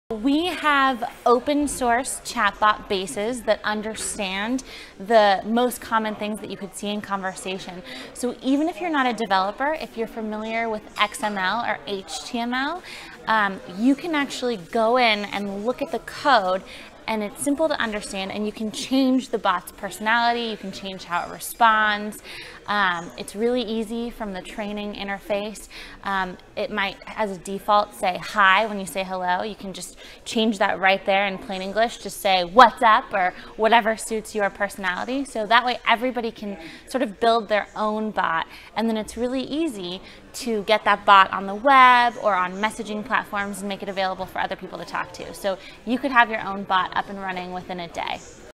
Comment construire un bot sur Pandorabots avec Lauren Kunze
Lauren Kunze, Principal chez Pandorabots explique comment construire et customiser son chatbot grâce aux ressources de sa plateforme Pandorabots.